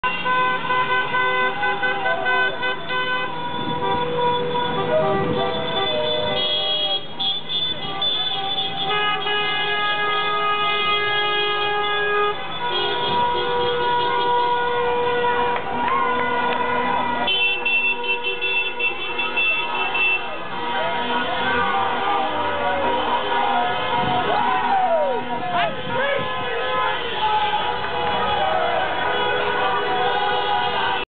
Italy World Champion - Gli Italiani verso Piccadilly Circus a Londra (1)
Appena vinto i Mondiali di Germania 2006 i tifosi italiani si apprestano ad invadere il centro di Londra.
A few minutes after Italian Team's victory in Berlin, Italian supporters move towards Piccadilly circus to Celebrate the World Championship of Italy.

Leonardo Rossetti